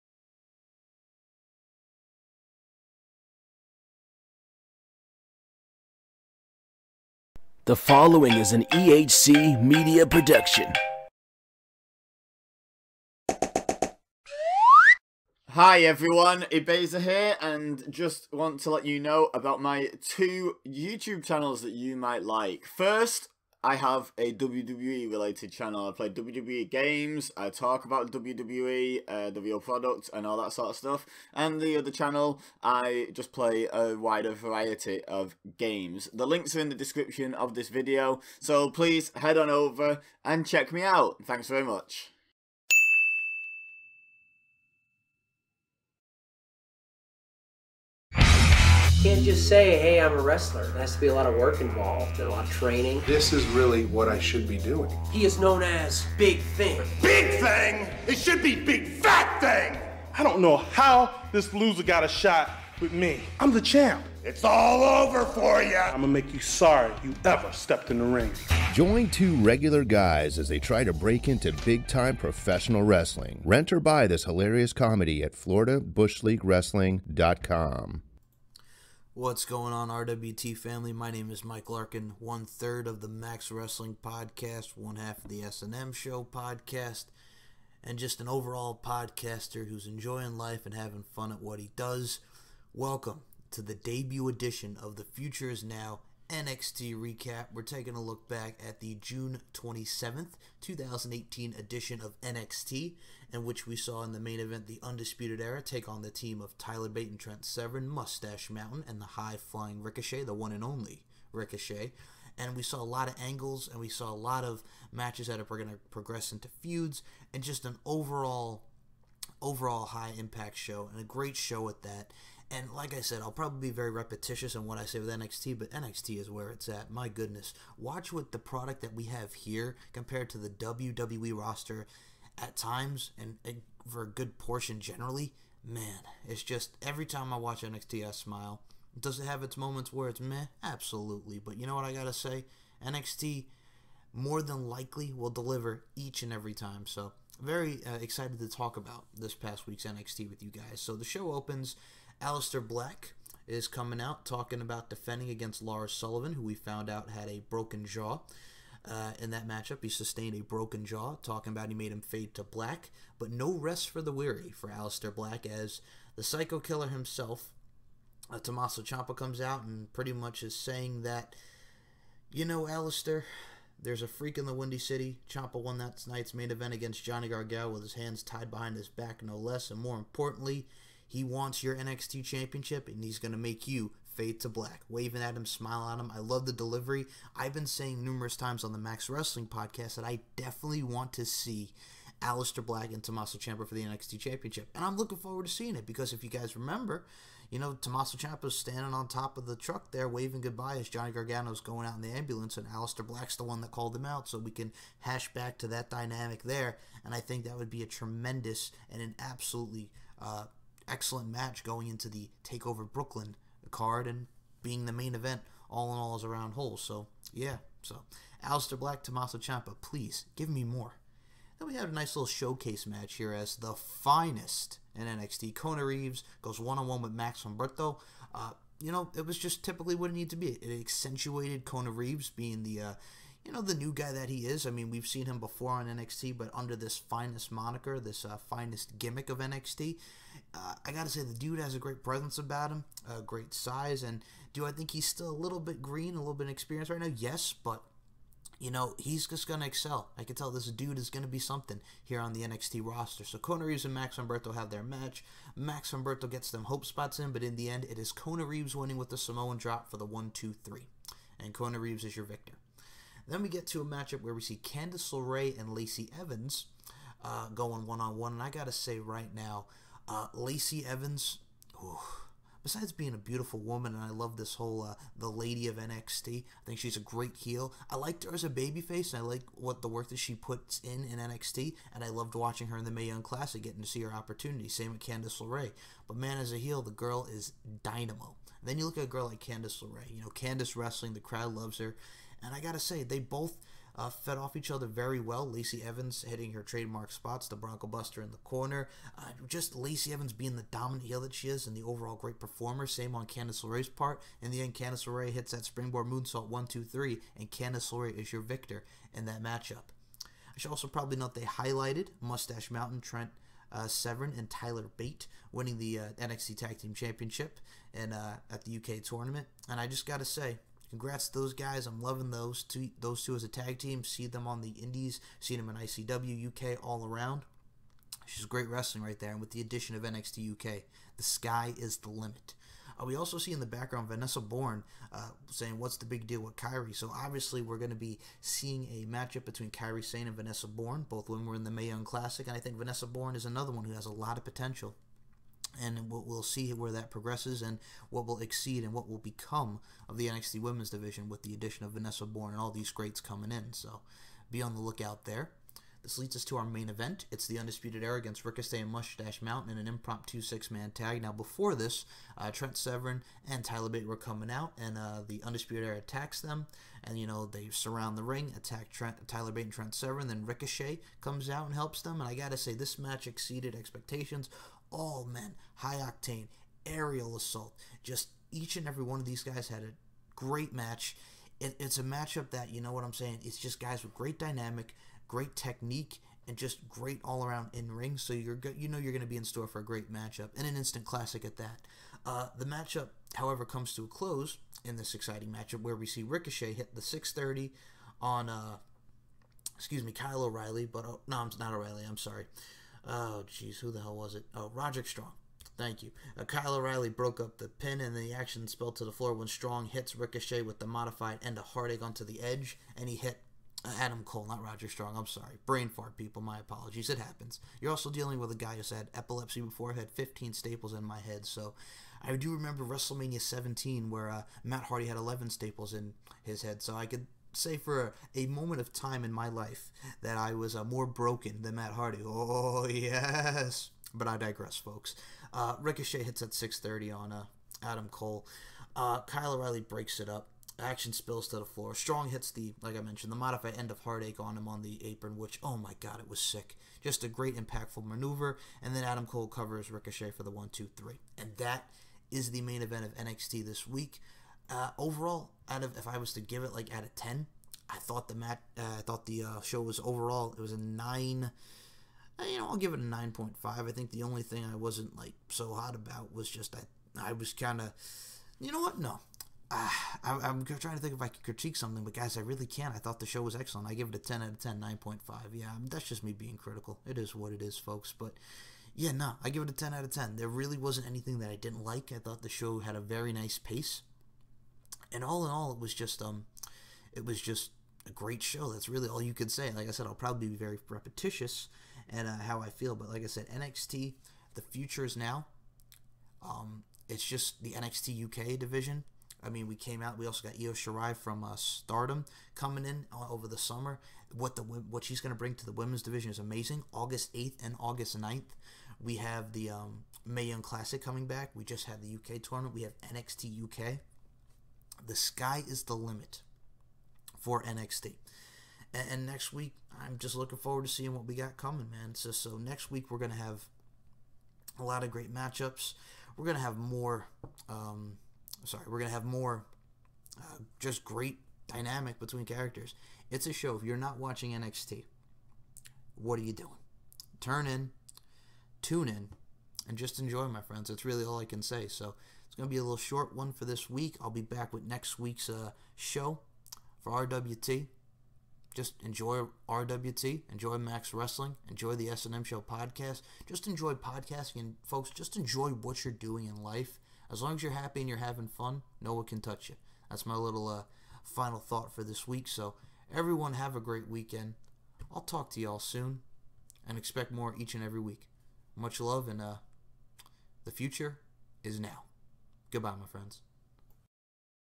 THE FUTURE IS NOW Ep1
Covering Nxt and 205 Live each week, Mike Larkin is here to show you that "THE FUTURE IS NOW"